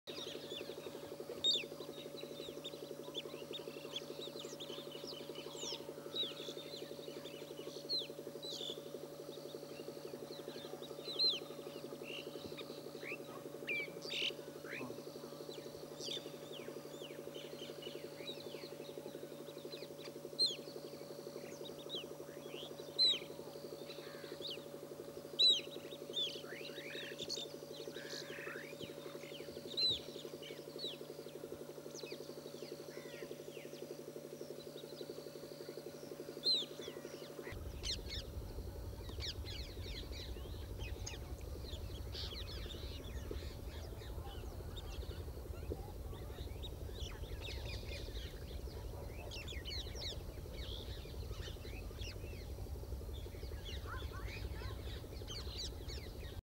Group of Black-winged Stilt wandering through water
Black-winged Stilt in Sariska.

According to Wikipedia: "The Black-winged Stilt or Common Stilt (Himantopus himantopus) is a widely distributed very long-legged wader in the avocet and stilt family (Recurvirostridae). Opinions differ as to whether the birds treated under the scientific name H. himantopus ought to be treated as a single species and if not, how many species to recognize. Most sources today accept 2--4 species."